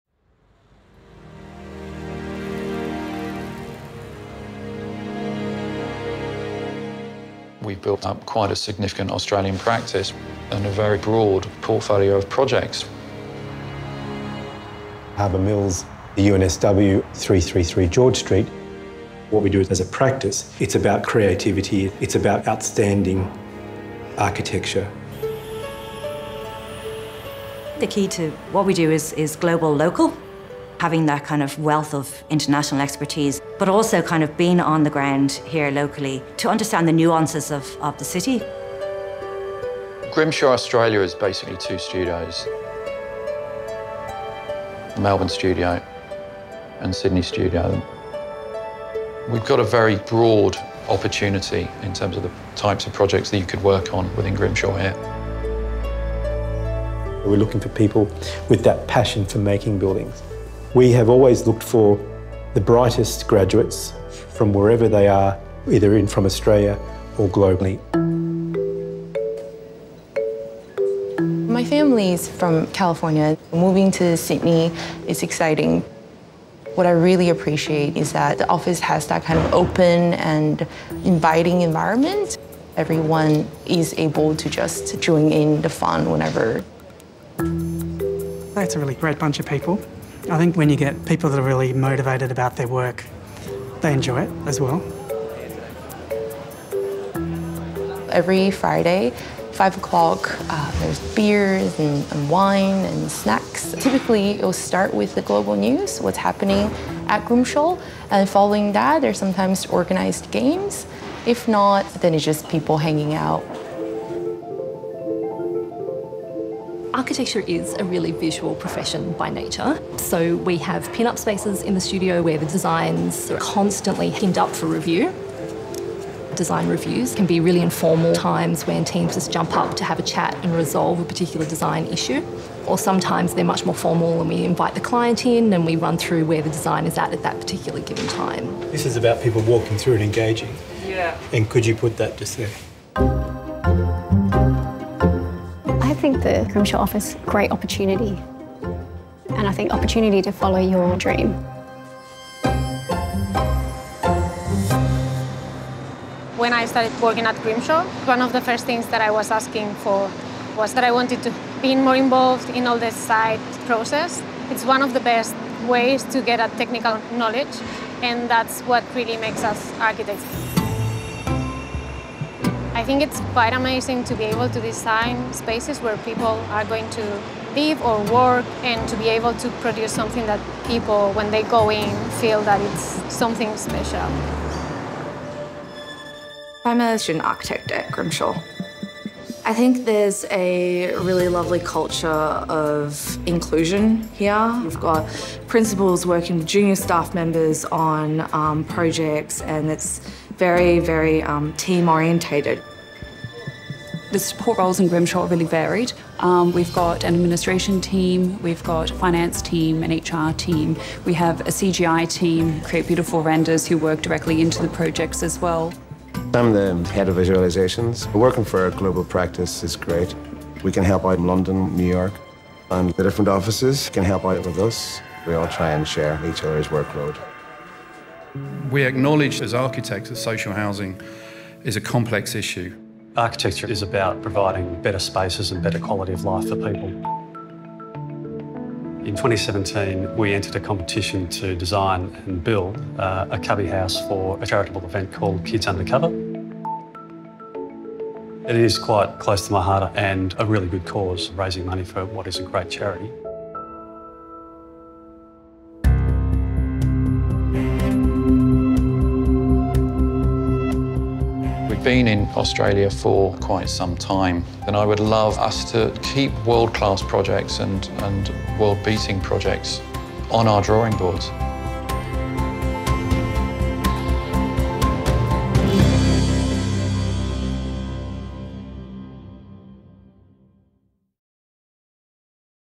Working at Grimshaw in Australia
Ever wondered what it's like to work in our Melbourne or Sydney offices? Find out about our vibrant work culture in this short Story!